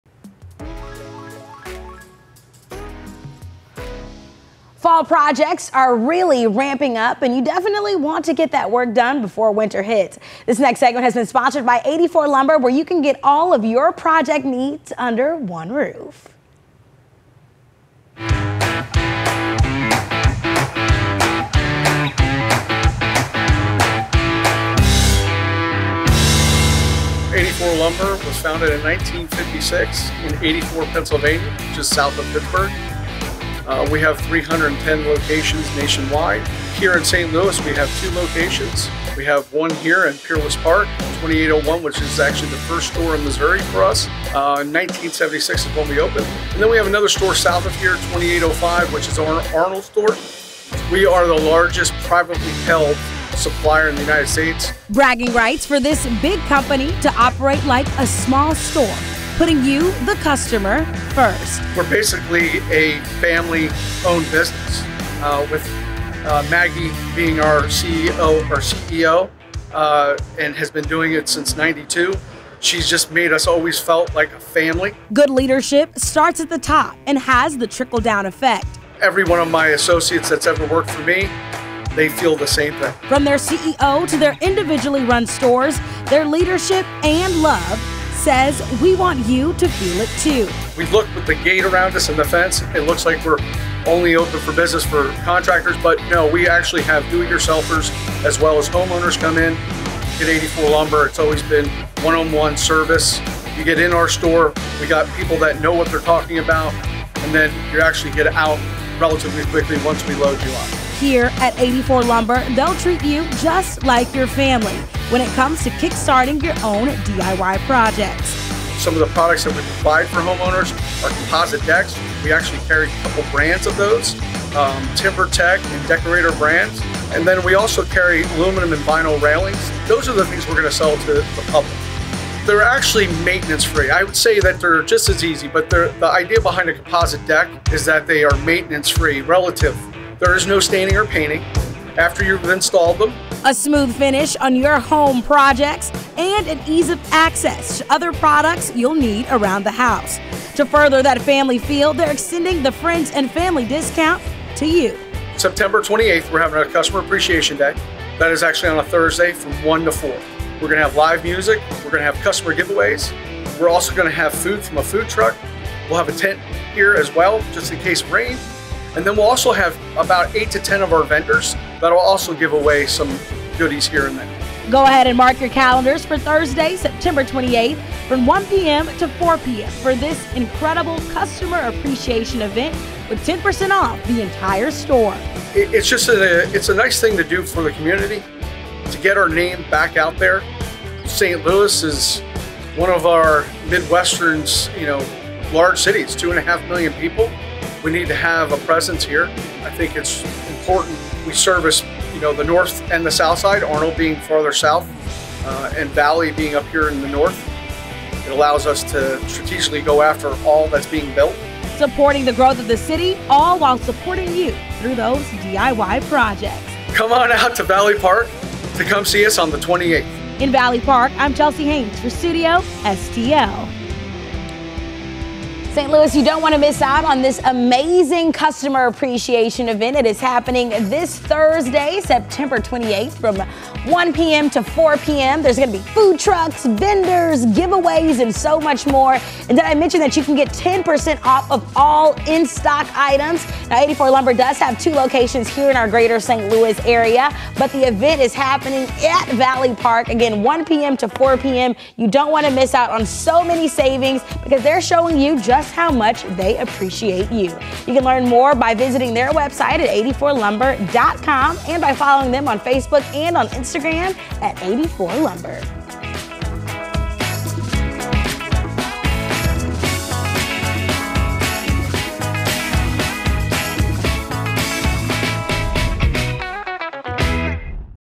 84 Lumber offering savings during customer appreciation event Thursday at their two local stores!
That fall project is only as good as the lumber that is used. Find the best lumber at 84 Lumber.

This week, on Thursday, September 28, there will be a customer appreciation event from 1-4 p.m. at Valley Park. Ask anyone of their in-store experts about what type of lumber to use or the best way to get that fall project started and finished before the cold weather hits.